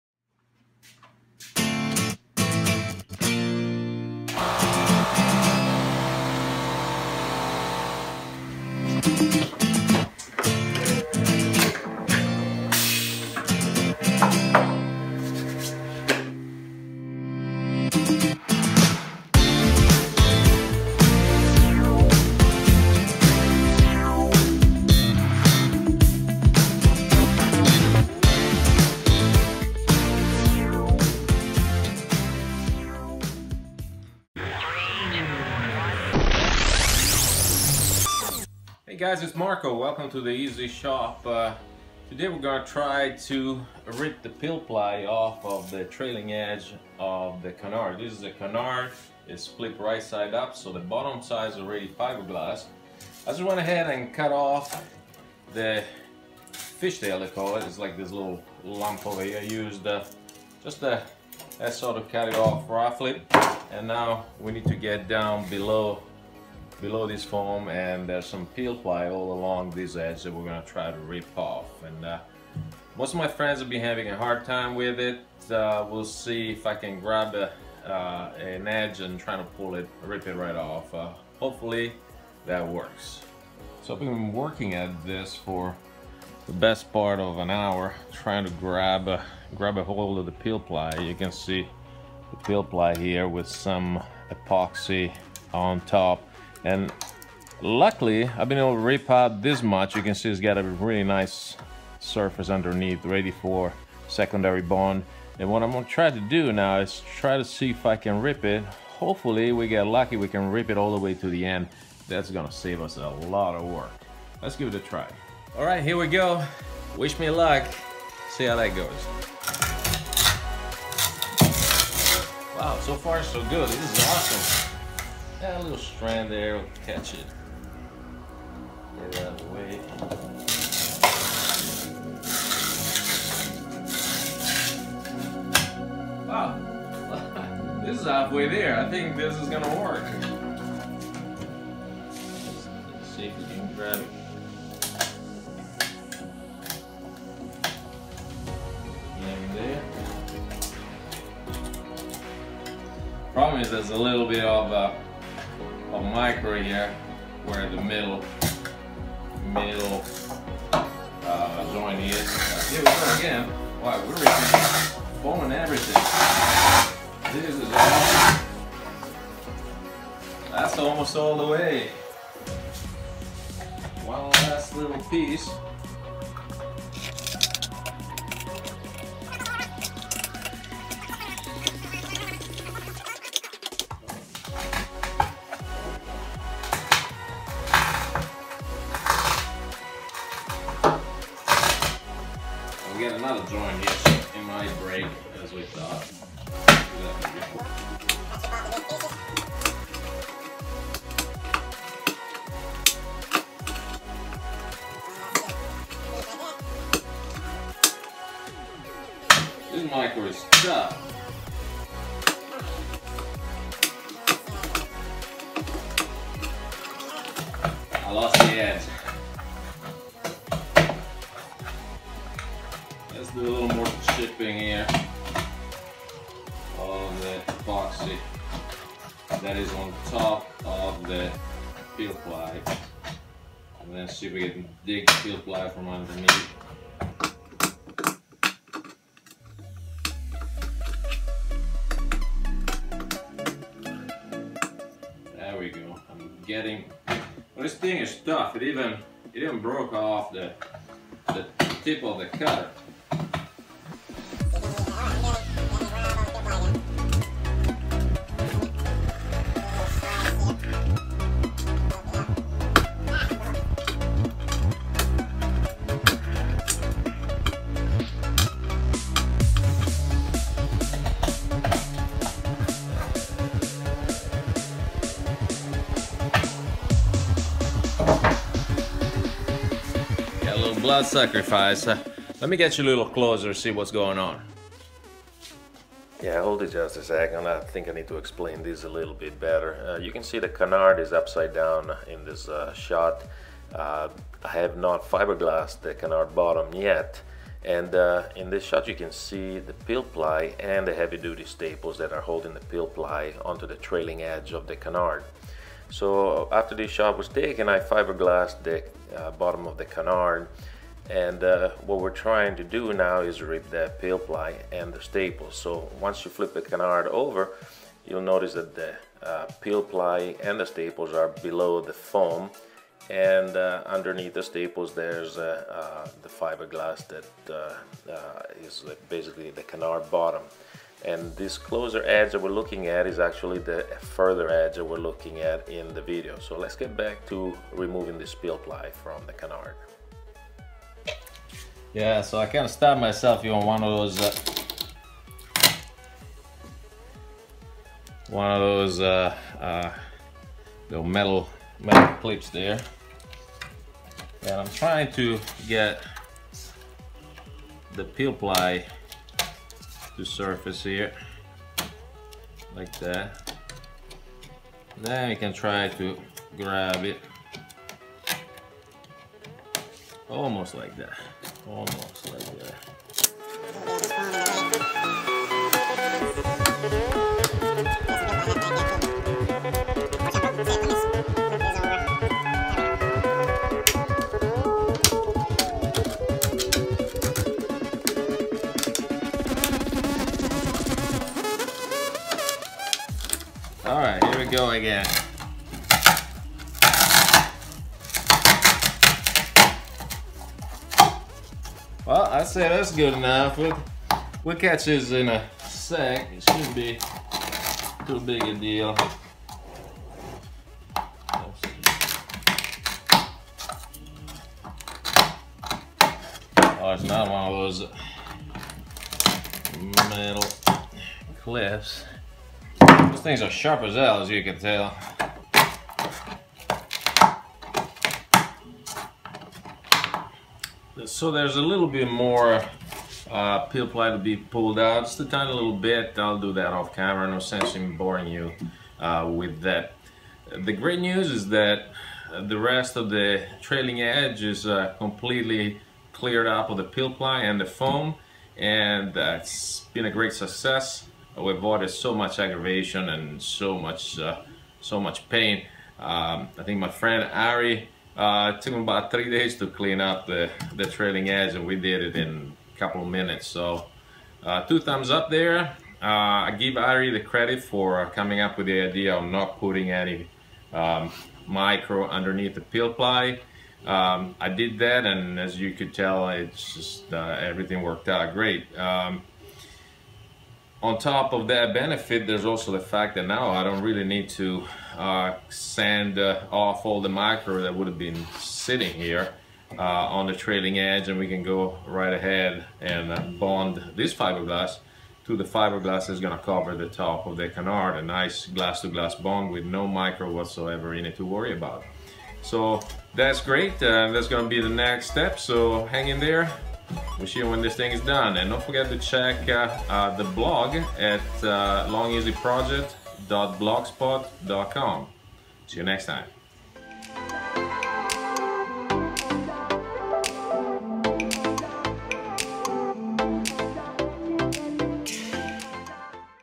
Trailing edge peel ply removal
Today I am removing the peel-ply from the trailing edge of the Long EZ's canard. Many of my friends have had a hard time doing this. Following Ary's theory, I made sure no microslurry made it under the peel-ply at glassing time. We shall see whether following this tip helped removing the peel-ply or not.